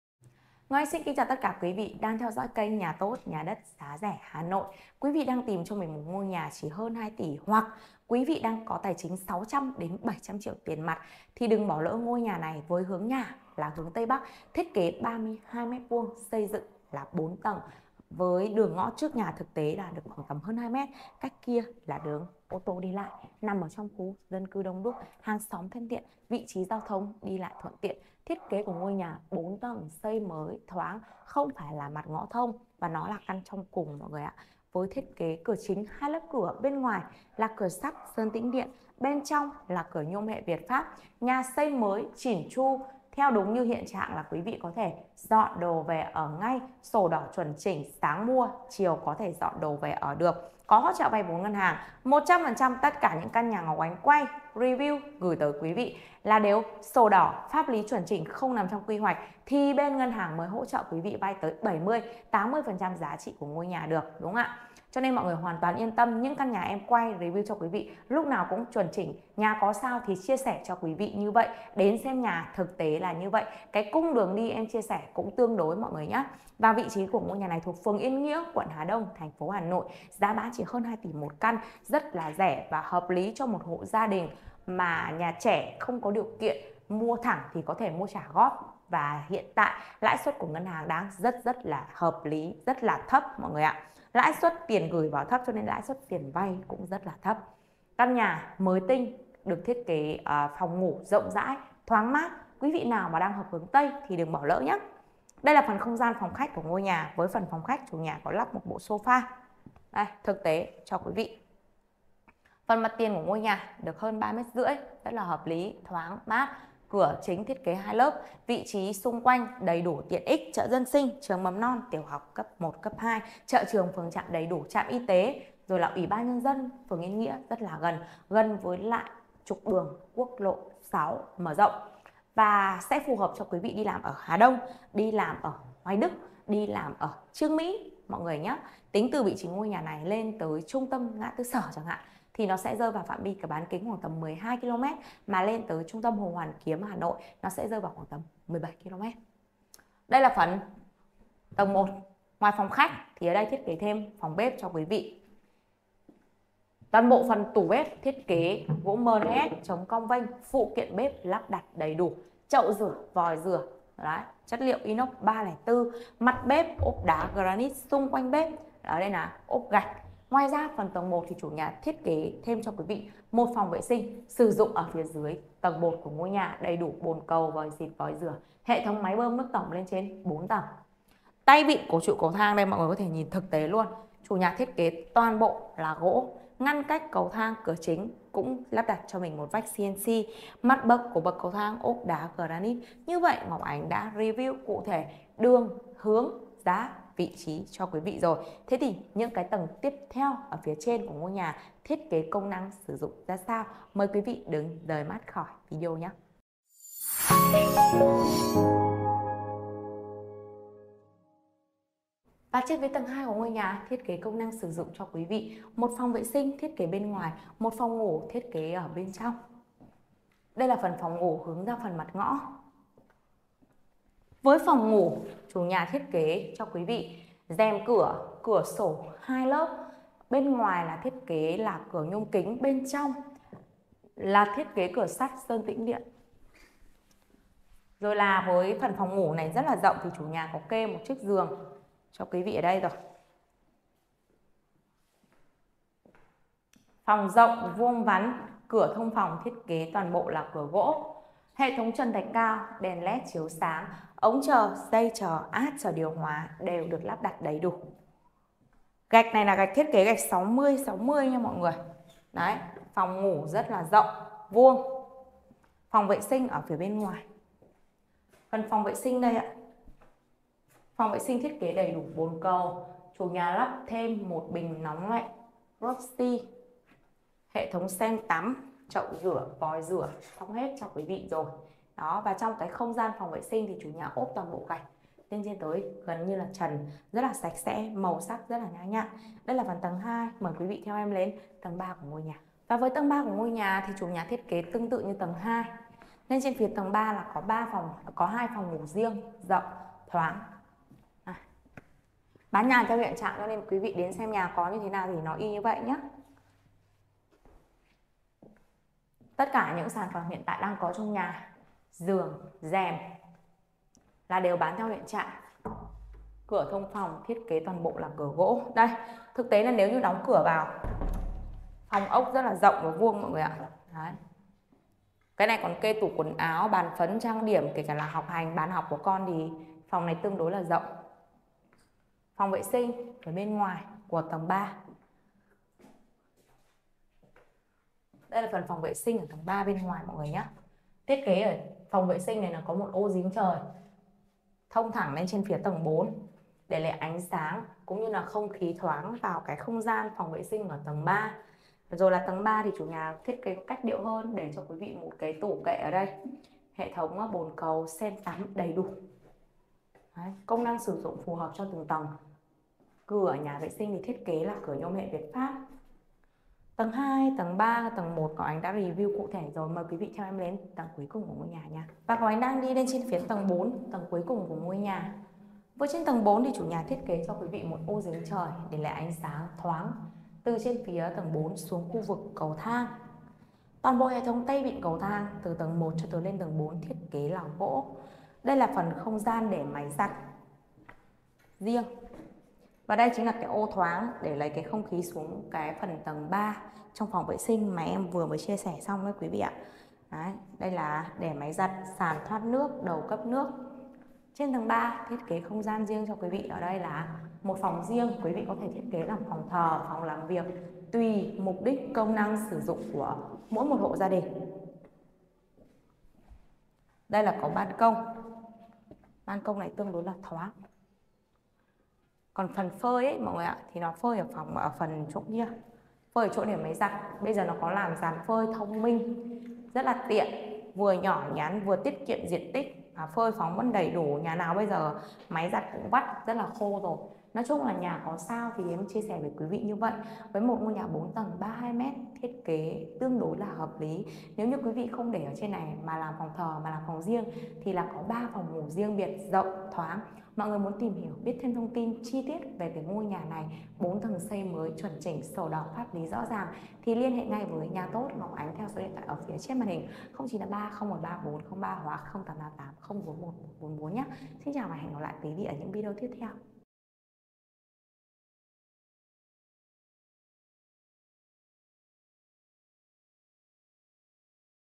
HƠN 2 TỶ - NHÀ QUẬN HÀ ĐÔNG XÂY MỚI 4 TẦNG 3 PHÒNG NGỦ, VỀ Ở NGAY | NHÀ TỐT
- Địa chỉ: Yên Nghĩa, Hà Đông, Hà Nội
- Diện tích: 32m2
- Hướng nhà: Tây Bắc
- Thiết kế: 4 tầng, 3PN, phòng bếp, khách, 3wc
- Giá bán: 2,15 tỷ (có thương lượng)